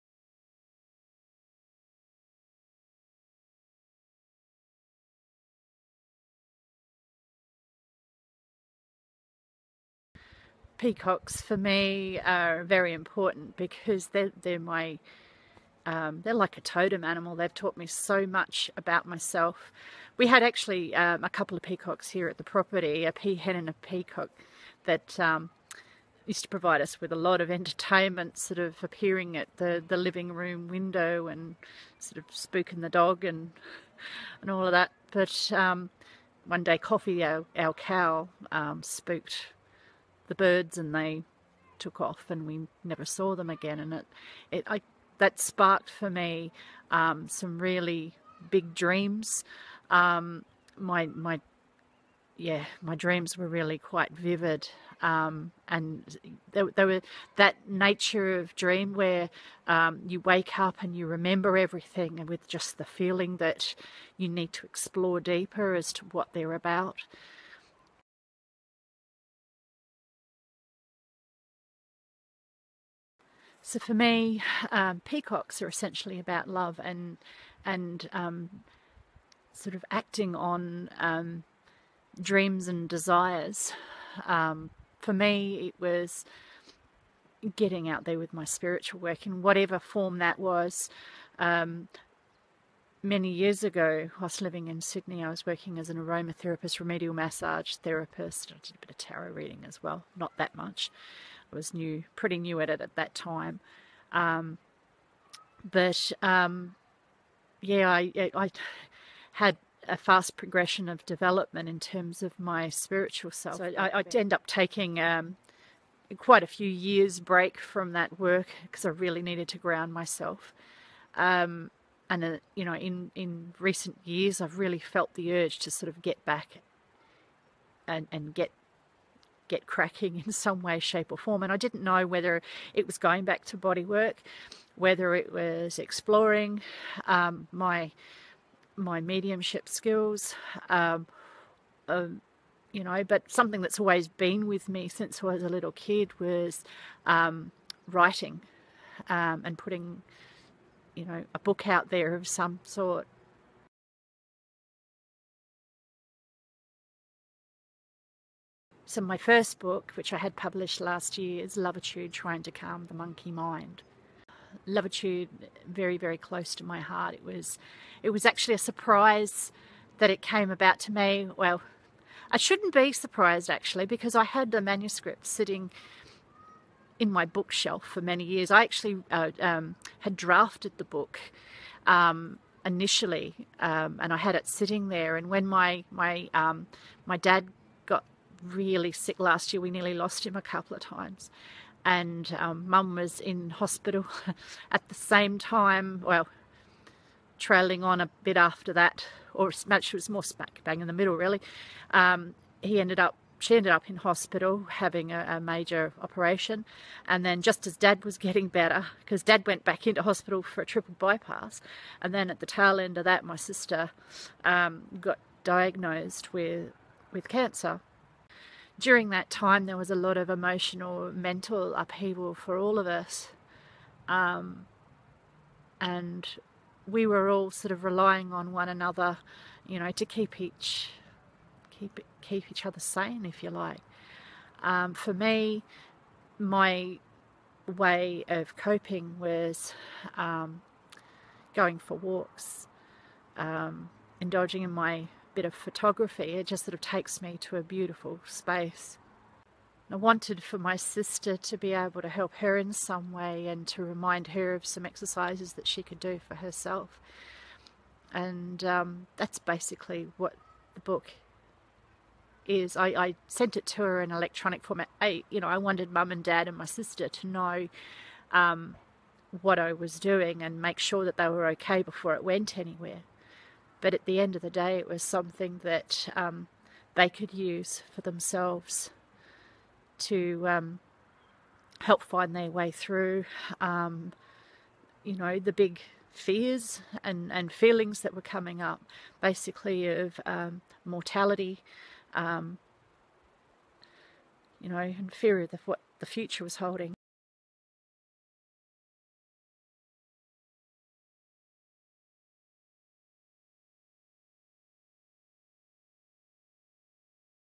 Peacock Dreaming and Lovitude
My first video (a nerve-wracking experience) talking a little about what Peacock Dreaming is for me and about my first book, Lovitude:  Trying To Calm The Monkey Mind.